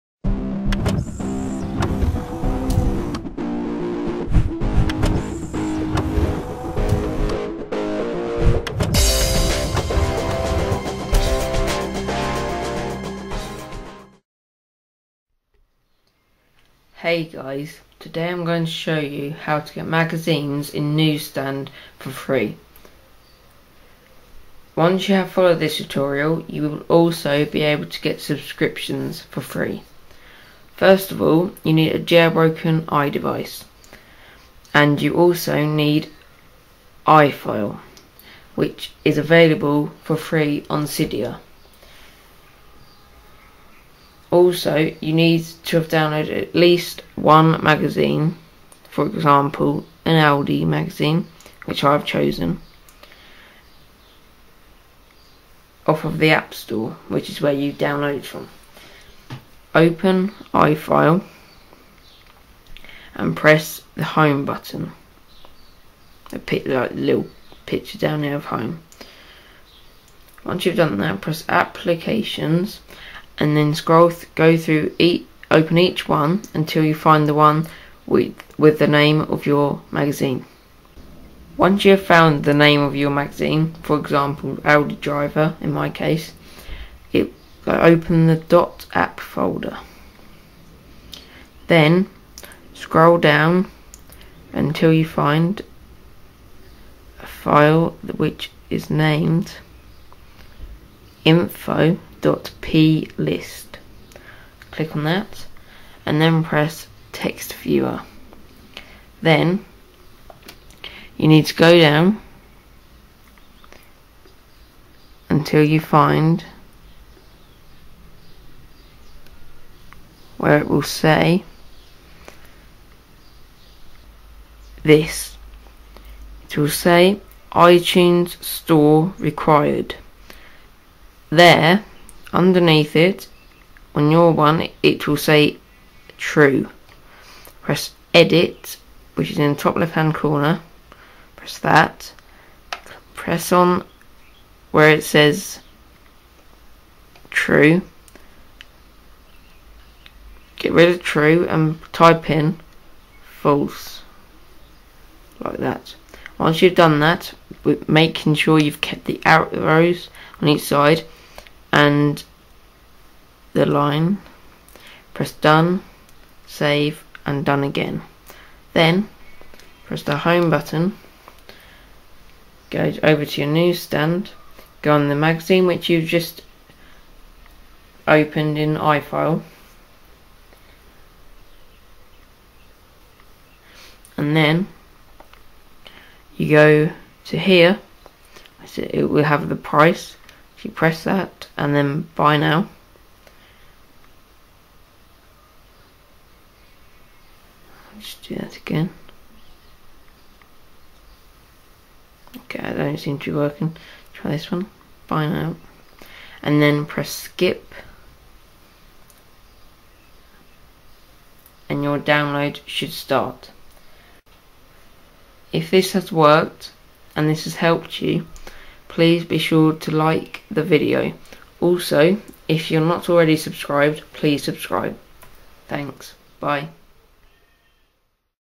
How To Get NewsStand Magazines For Free
This tutorial will show you how to get magazines on iDevices for free.